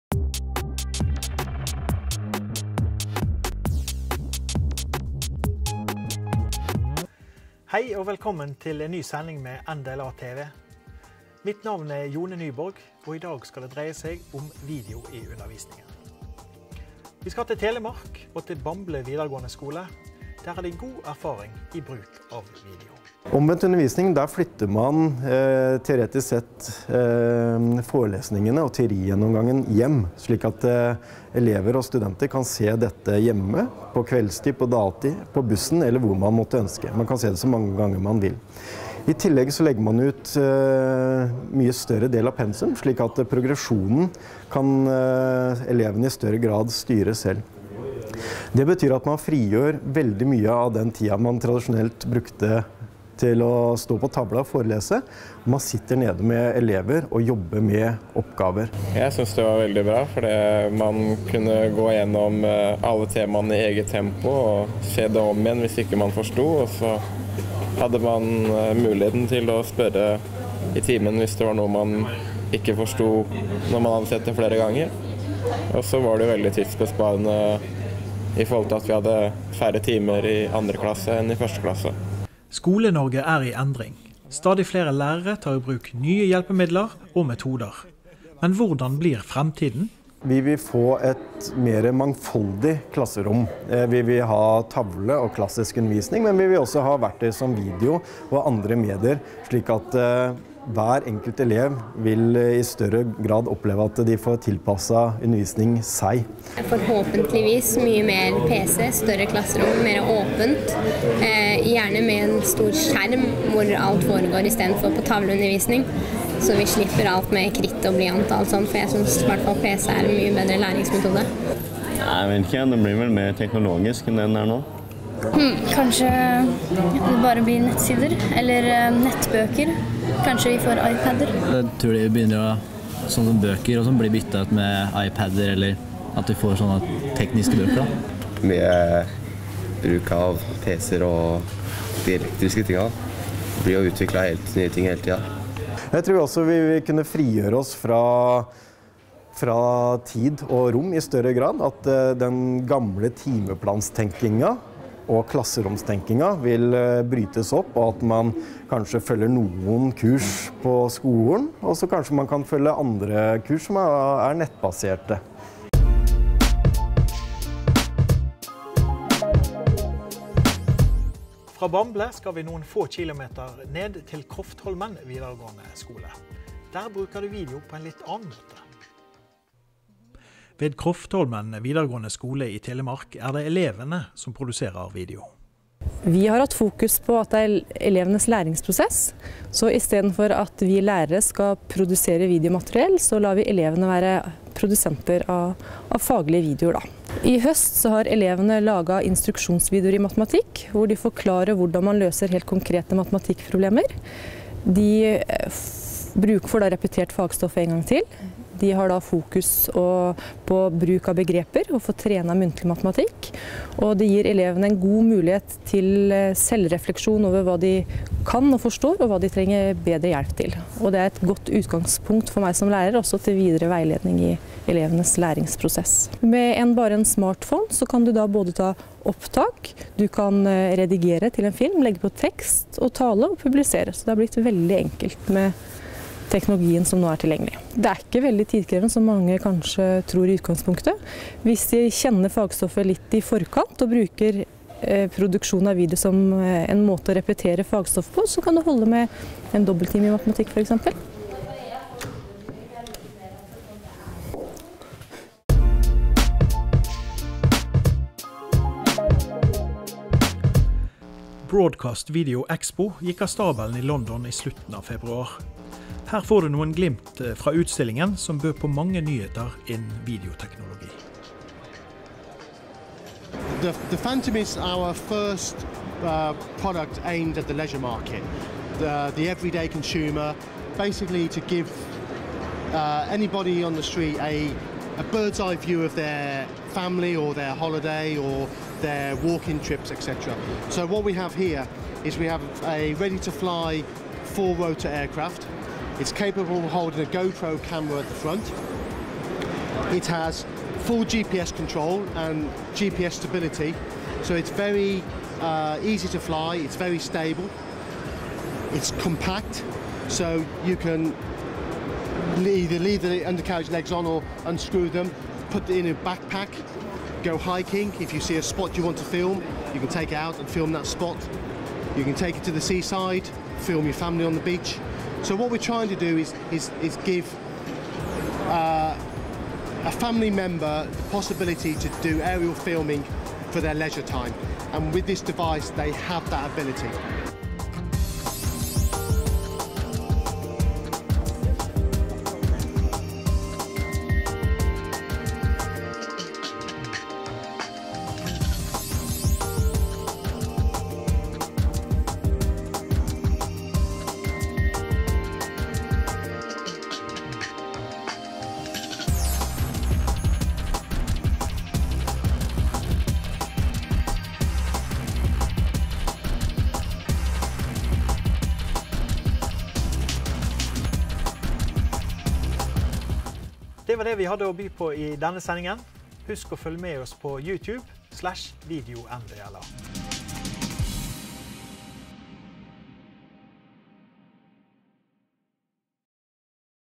Video i skolen. Omvendt undervisning. Elevproduserte videoer. Glimt fra BVEXPO. | NDLA TV 3. sending
Tredje sending i NDLA TV. Tema er video i skolen. Vi byr på reportasjer fra Bamble vgs, Croftholmen vgs og BVexpo.com.

Stor takk til:
Tom Jarle Christiansen, Linda Therese Frønes, Rune Mathisen, Eivind Bergerud Pettersen, Elisabeth Holien, Jaran Godejord, Borgar Elle, Linnea og Aleksander.

Reporter: Jone Nyborg. Foto: Odd Magne Myre.

Regi: Brita Sørli Jærnes
Lyd og lys: Håvard Bekkestad
Foto og bildemiks: Odd Magne Myre og Håvard Bekkestad
Redigering og etterarbeid: Gunstein Myre
Programleder og manus: Jone Nyborg

Produsert hos Sanden Media i Kristiansand i samarbeid med Artpilot.